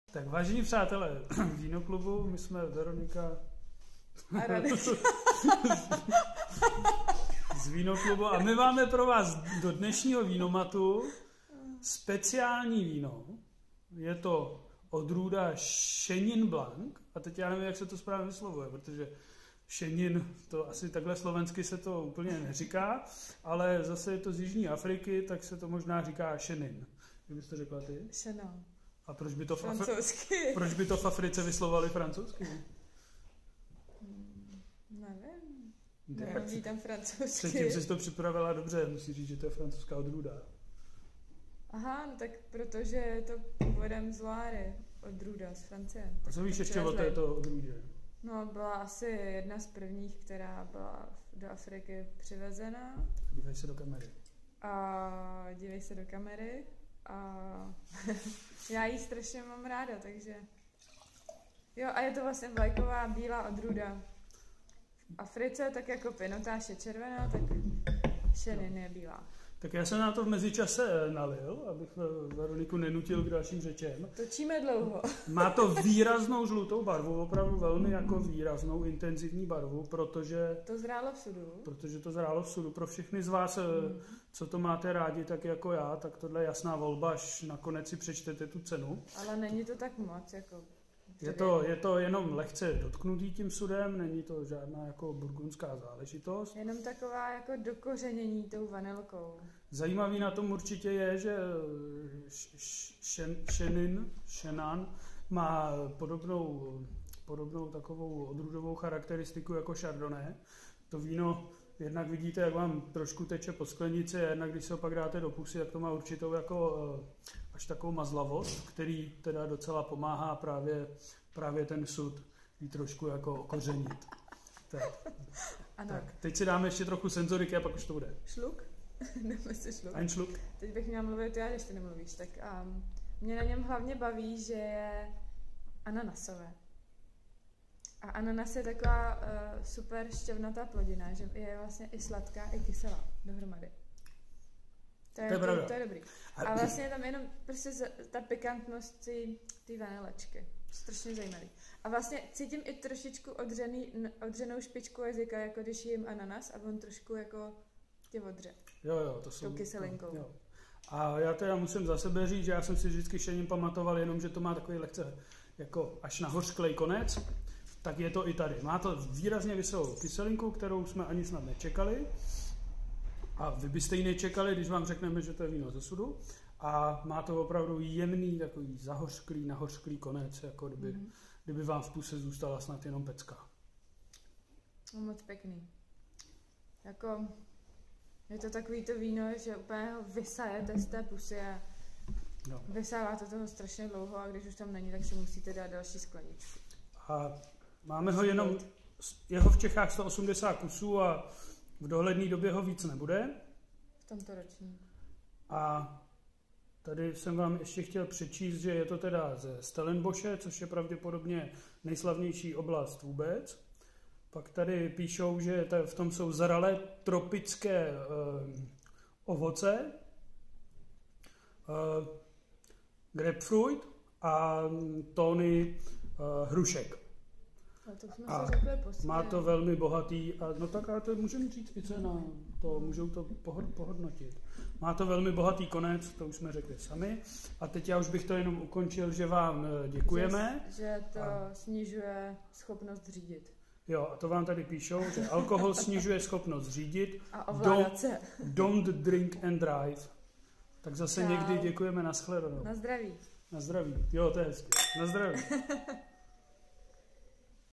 Video-degustace: Stellenrust - Chenin blanc, 2014
Radek a Veronika z Víno-klubu představují jihoafrické víno Chenin blanc ;-)